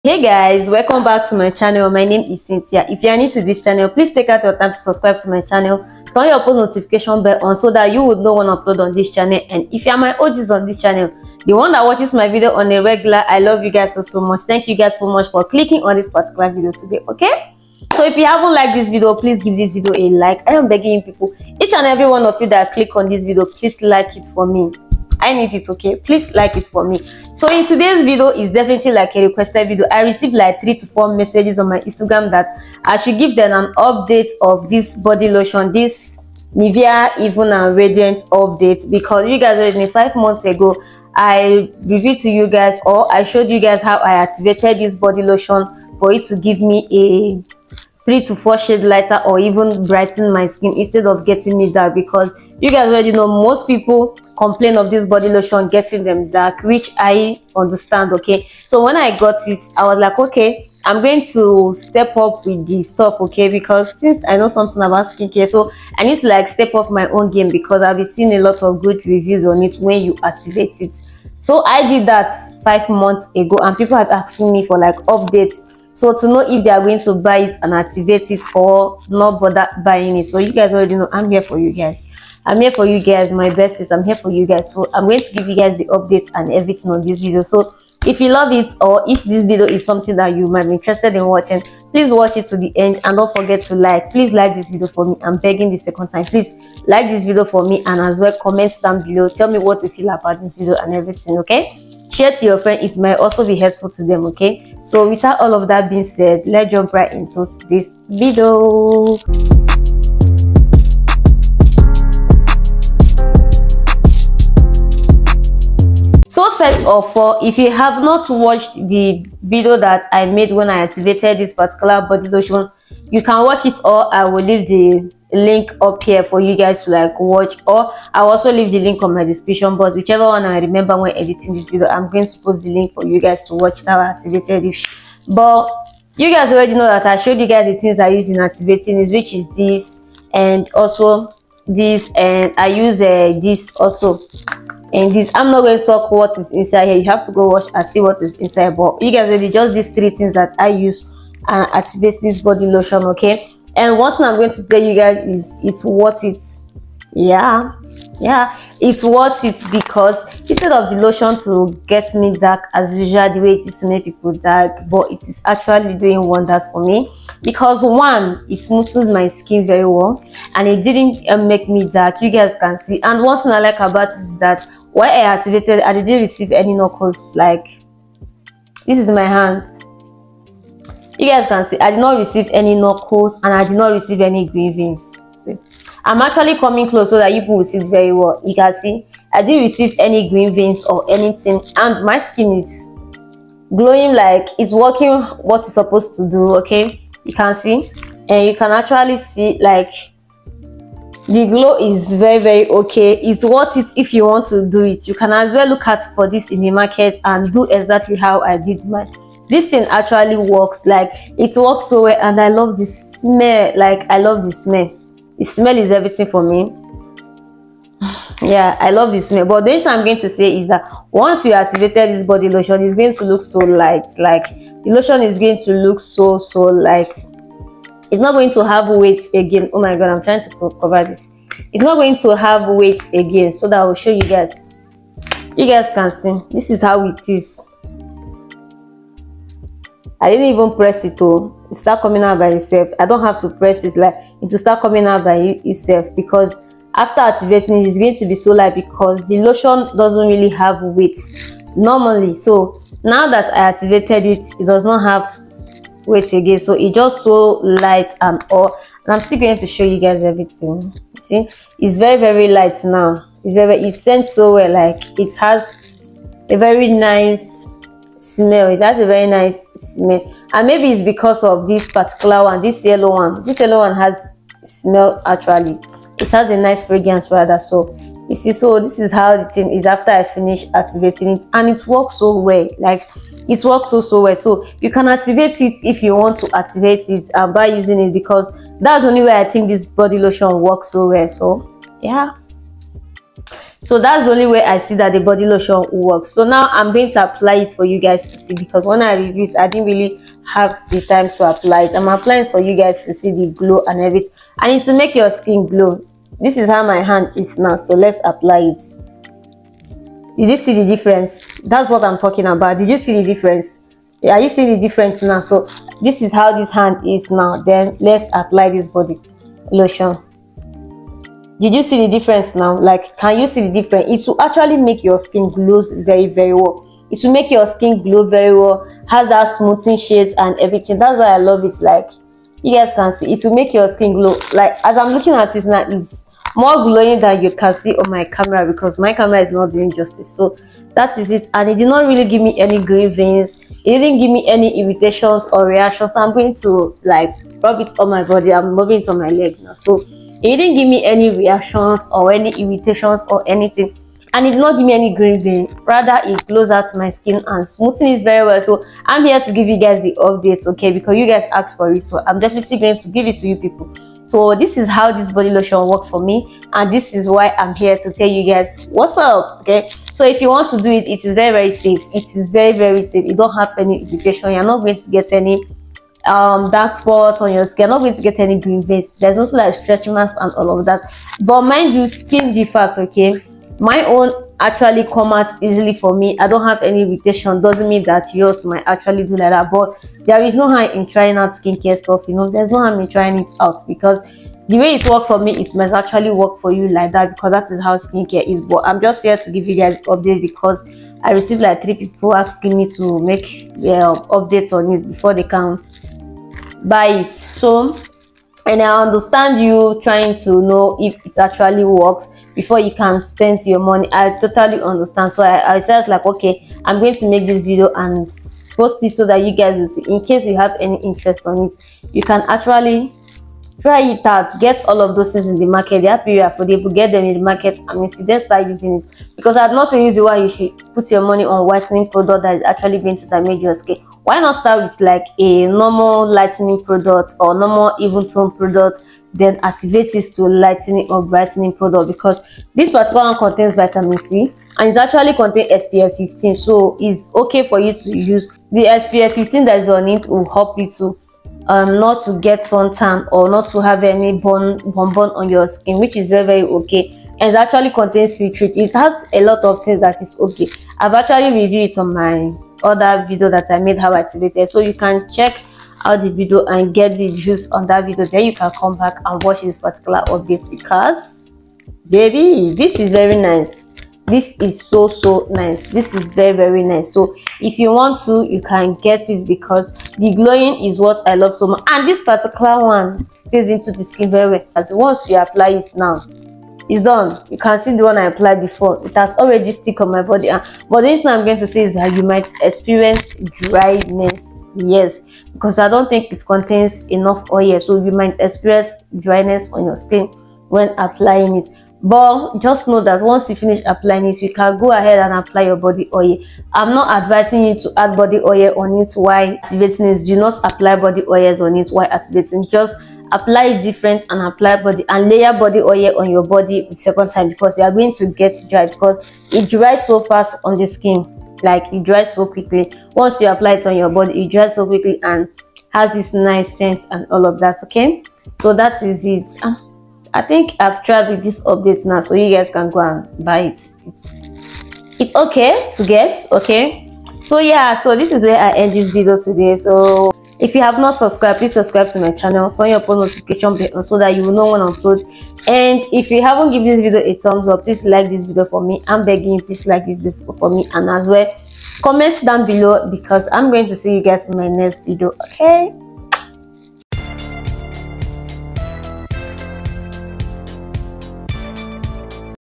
HOW TO USE NIVEA EVEN AND RADIANT LOTION TO ACHIEVE 3-4 SHADES LIGHTER | glows and brightens.
Hello my besties 👯‍♀️

This video contains update on nivea even and radiant lotion, how it works and my own review after using it for 5months ,

Kindly
Share to your friends too